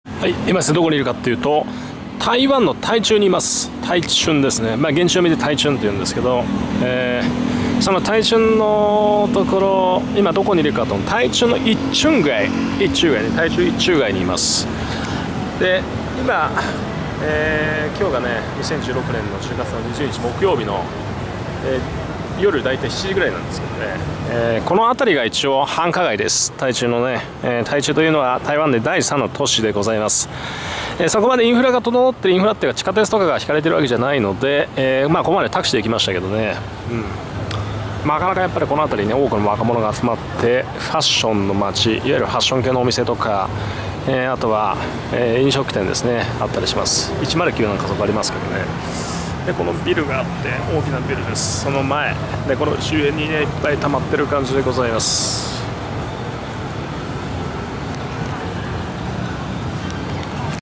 アキーラさん散策①台湾・台中・台中一中街！Taichung city in Taiwan
台中の繁華街である台中一中街に潜入。ファッション関連のお店や飲食店、屋台が多く集まる場所です。台中に来たら訪れるべき場所です。

平成２８年(２０１６年）１０月に２週間の日程で台湾を周遊、
台北⇒台中⇒嘉義⇒台南⇒福瀬村⇒高雄⇒台東⇒花蓮⇒太魯閣⇒台北と１周しました。